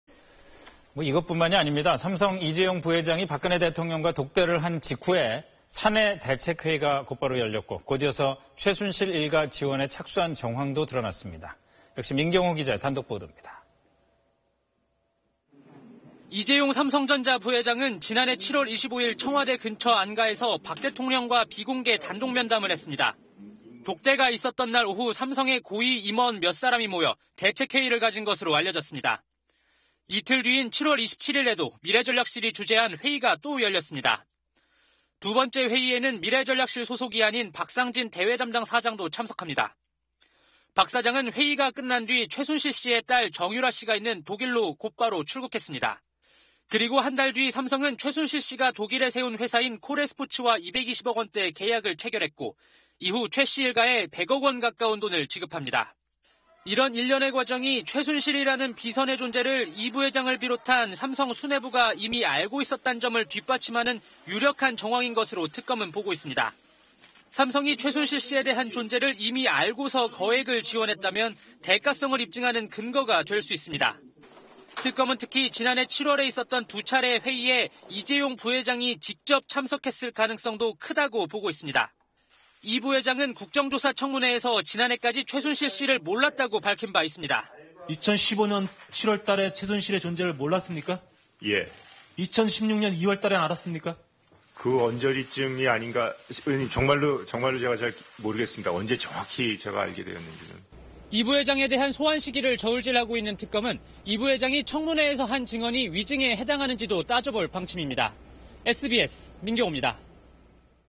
[단독] 대통령-이재용 독대 직후…삼성 '긴급 간부회의' / SBS
이것뿐만이 아닙니다. 삼성 이재용 부회장이 박근혜 대통령과 독대를 한 직후에 사내 대책회의가 곧바로 열렸고, 곧이어서 최순실 일가 지원에 착수한 정황도 드러났습니다.

역시 민경호 기자의 단독 보도입니다.